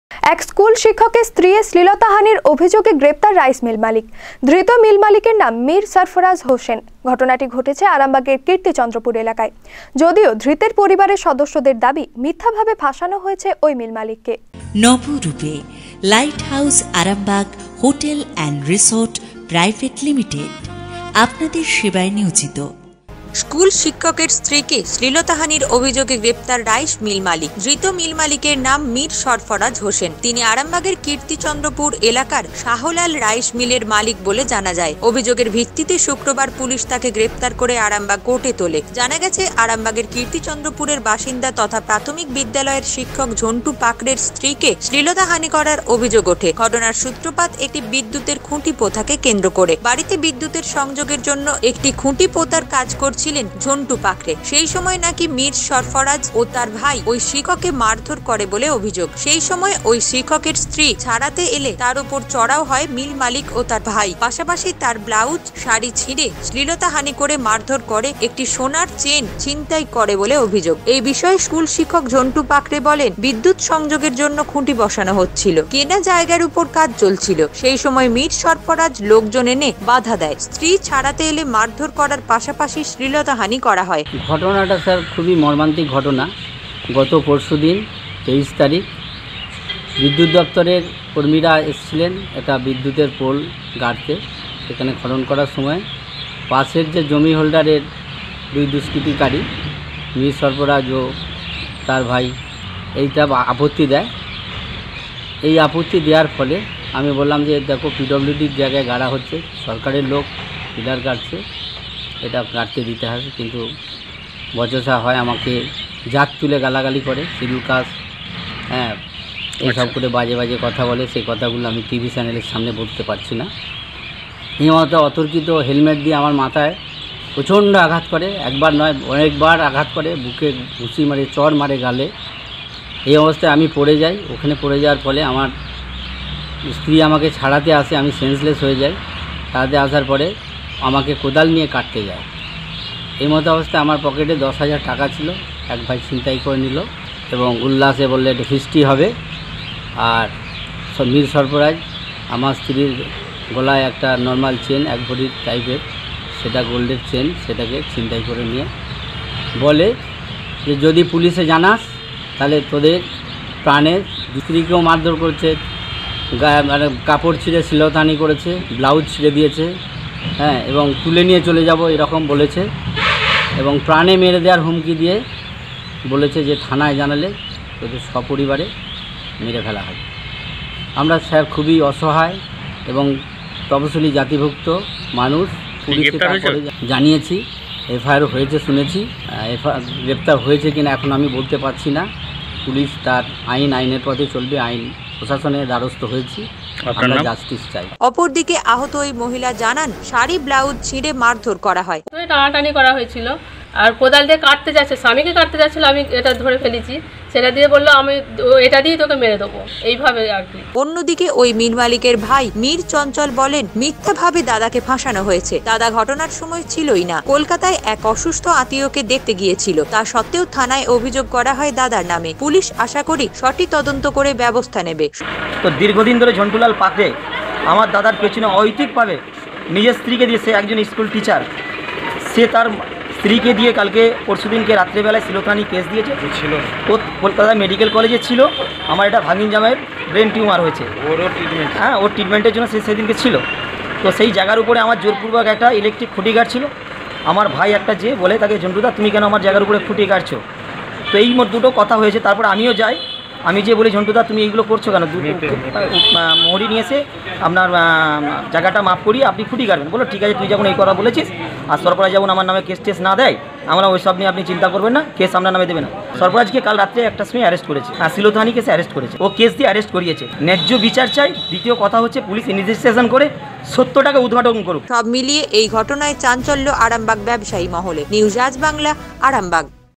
বধূর শ্লীলতাহানি
স্কুল শিক্ষকের স্ত্রীকে শ্লীলতাহানির অভিযোগে গ্রেফতার রাইস মিলের মালিক। অবশেষে জামিন পেলেন মিল মালিক